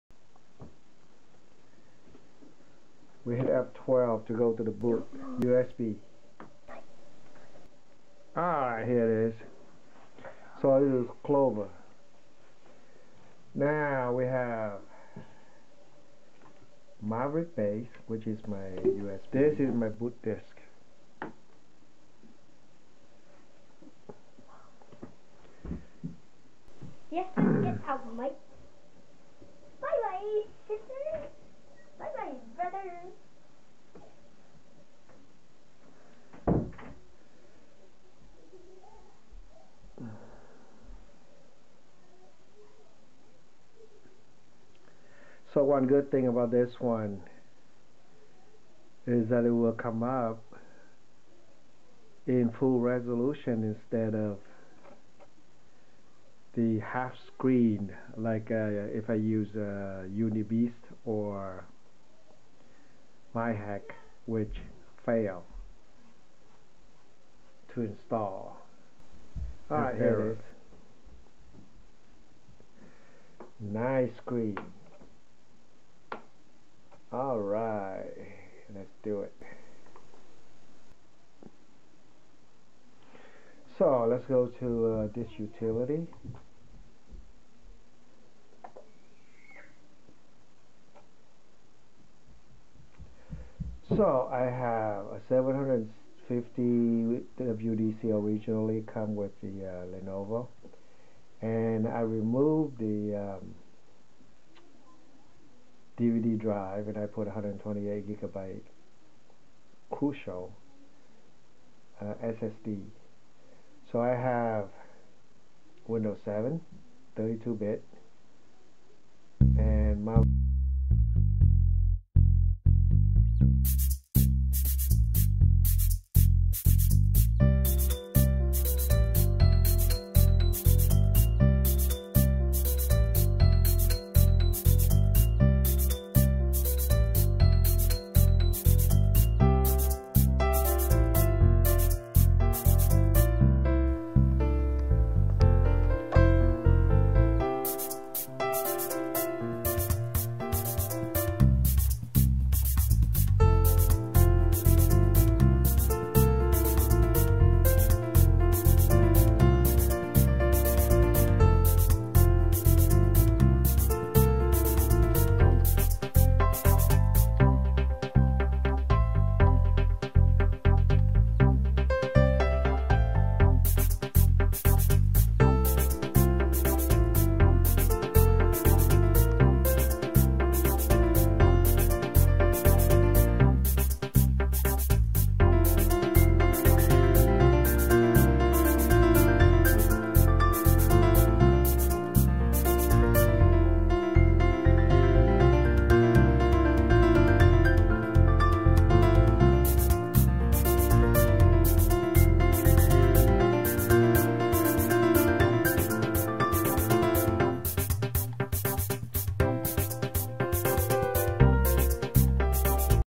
Install Mavericks On Lenovo G770 i5 HD3000 using Clover
go to the page to see the links for config.plist and kexts for Mavericks